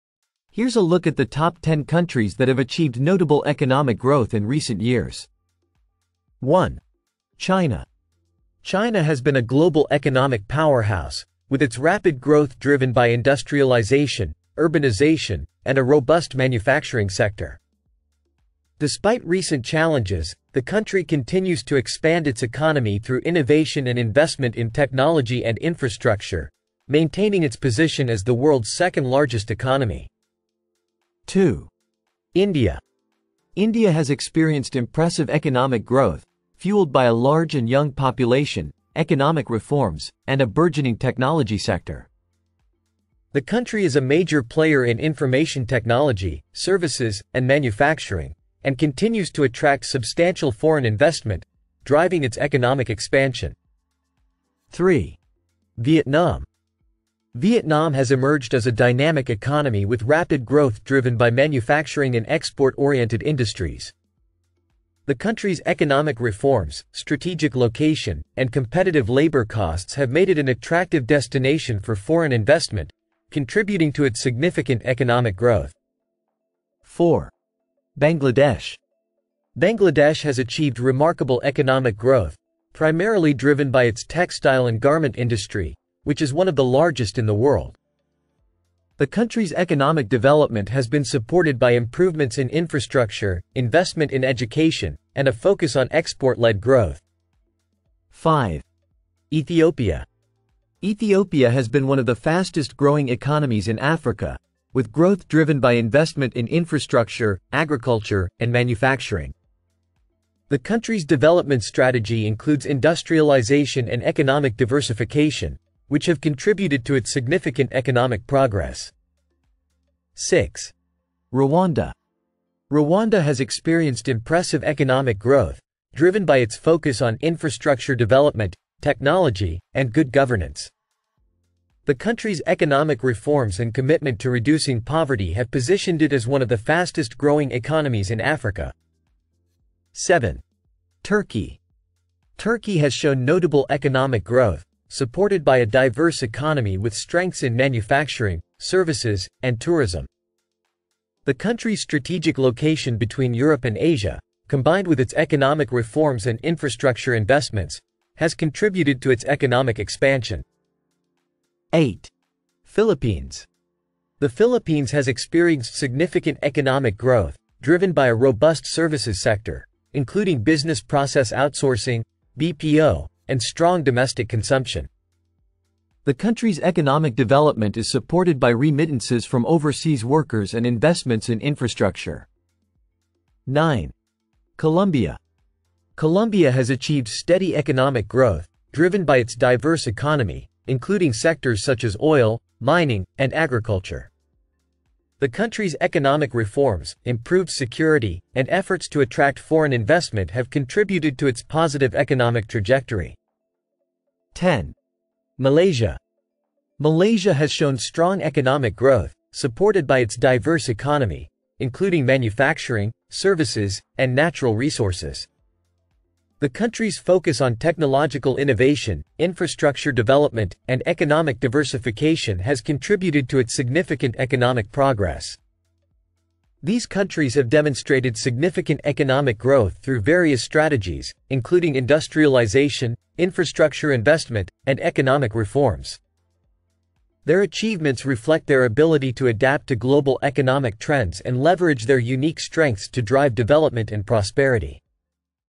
Top 10 Countries Achieving Significant Economic Growth
Discover the top 10 countries experiencing significant economic growth. From emerging markets to developed economies, these nations are seeing remarkable increases in economic performance and development. Watch now to learn about the countries leading in economic progress!